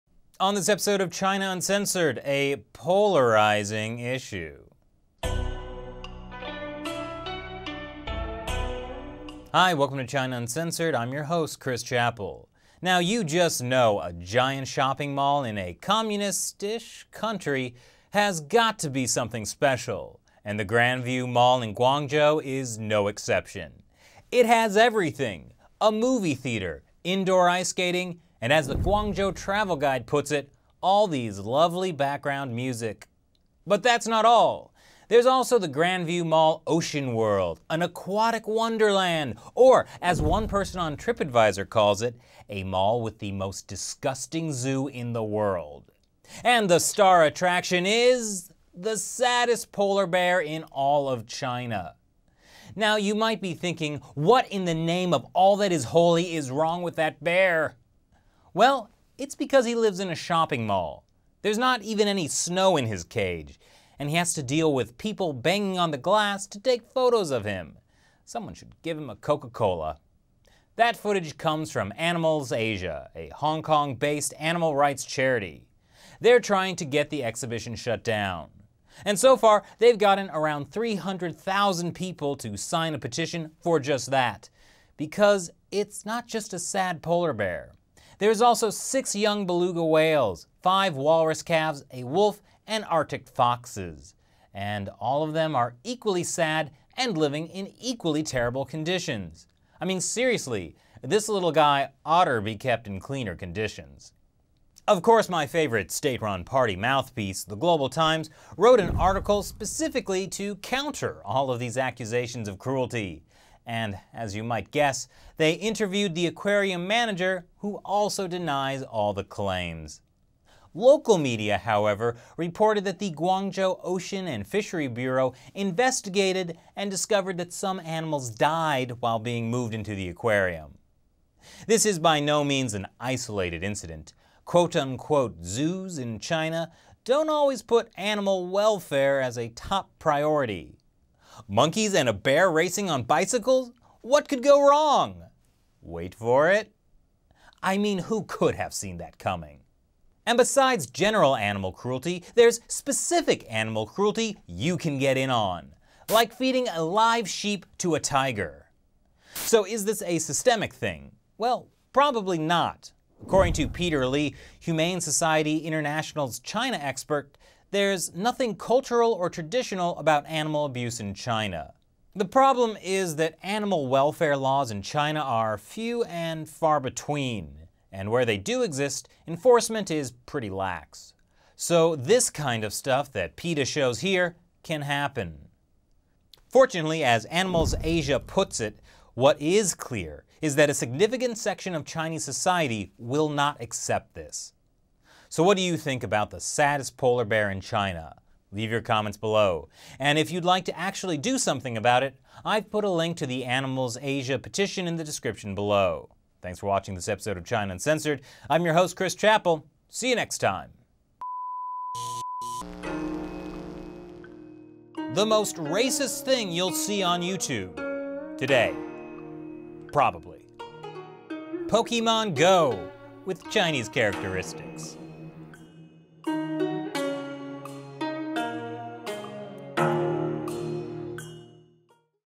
The Saddest Polar Bear in China
The world's saddest polar bear is stuck living in a mall in China. And he's just one of many animals suffering the same miserable fate inside this Chinese mall.

Contribute! Join the China Uncensored 50-Cent Army!